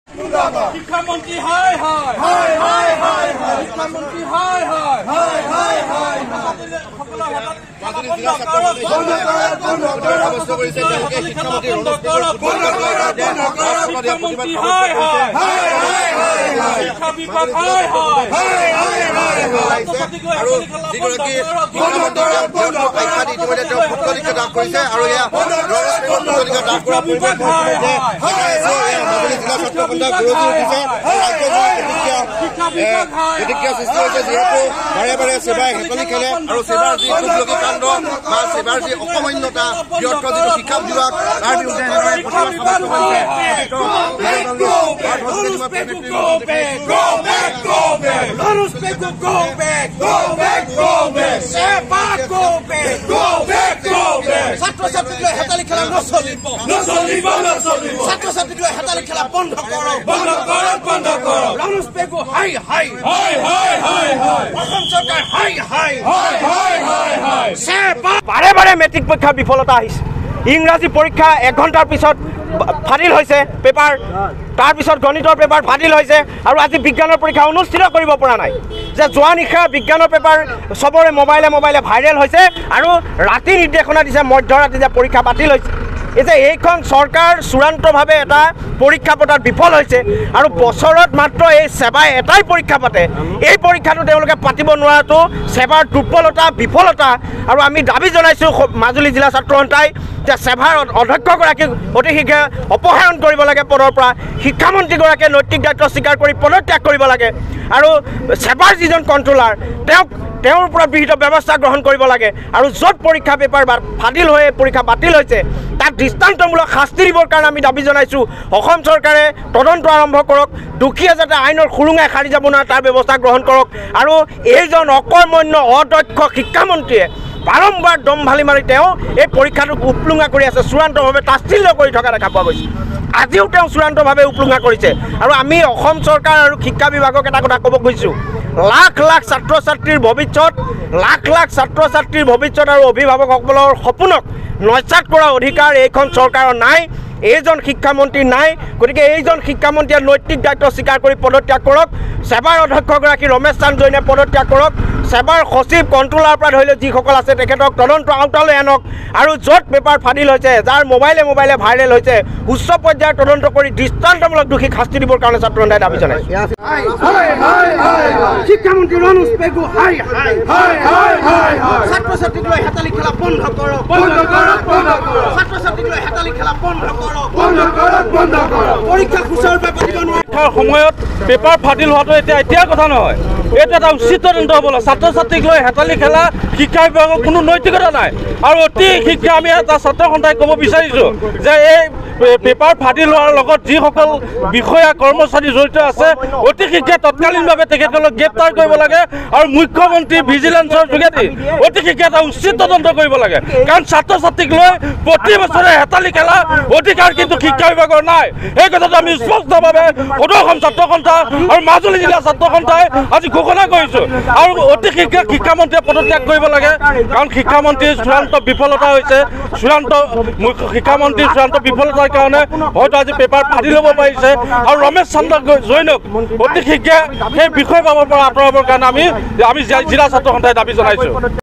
মাজুলীত শিক্ষামন্ত্ৰীৰ প্ৰতিকৃতি দাহ আছুৰ :মেট্ৰিকৰ প্ৰশ্নকাকত ফাদিল
মাজুলীত শিক্ষামন্ত্ৰীৰ প্ৰতিকৃতি দাহ আছুৰ

  শিক্ষা মন্ত্ৰী ডা° ৰণোজ পেগু নেতৃত্বত শিক্ষাবিভাগে ছাত্ৰ-ছাত্ৰী সকলক লৈ চূড়ান্ত হেতালি খেলা কাৰ্য অতি দুৰ্ভাগ্যজনক৷ আছুৰ ৰাজ্যিক কাৰ্যনিৰ্বাহক পবিত্ৰ গোস্বামীয়ে কয় যে- বছৰত মাত্ৰ এটা পৰীক্ষাকে গোপনীয়তা ৰক্ষা কৰি নিয়াৰিকৈ অনুষ্ঠিত কৰিব নোৱাৰা ছেবা গৰিহণা আৰু ধিক্কাৰৰ যোগ্য৷ সদৌ মাজুলী জিলা ছাত্ৰ সন্থাৰ সভাপতি অজিত হাজৰিকা আৰু সাধাৰণ সম্পাদক বিতোপন শৰ্মাই কয় যে- ১৩মাৰ্চৰ বিজ্ঞানৰ পৰীক্ষা বাতিল কৰিব লগা হোৱা পৰিস্থিতিৰ বাবে দায়ী কোন? নৈতিক দায়িত্ব স্বীকাৰ কৰি ডা° ৰণোজ পেগু আৰু ছেবাৰ অধ্যক্ষ গৰাকীয়ে পদত্যাগ কৰা উচিত৷ মুখ্যমন্ত্ৰীৰ প্ৰত্যক্ষ হস্তক্ষেপত দোষীক দৃষ্টান্তমুলক শাস্তিৰ দাবী জনালোঁ৷ আইনৰ সুৰুঙাৰে যাতে কোনো সাৰি যাব নোৱাৰে তাৰ ব্যৱস্থা গ্ৰহণ কৰিবলৈ দাবী জনাইছে মাজুলী আছুৱে৷ কোনো এটা বছৰ এই মেট্ৰিক পৰীক্ষা সুকলমে অনুষ্ঠিত কৰা দেখা নগ’ল৷ এই চৰকাৰ আৰু শিক্ষা বিভাগৰ চূড়ান্ত বিফলতা৷ আজি জিলা সদৰ গড়মুৰত মাজুলী জিলা ছাত্ৰ সন্থাই শিক্ষামন্ত্ৰী আৰু ছেবাৰ প্ৰতিকৃতি দাহ কৰি প্ৰতিবাদ সাব্যস্ত কৰাৰ লগতে শিক্ষামন্ত্ৰী আৰু ছেবাৰ অধ্যক্ষৰ দাবী পদত্যাগৰ দাবী তোলে৷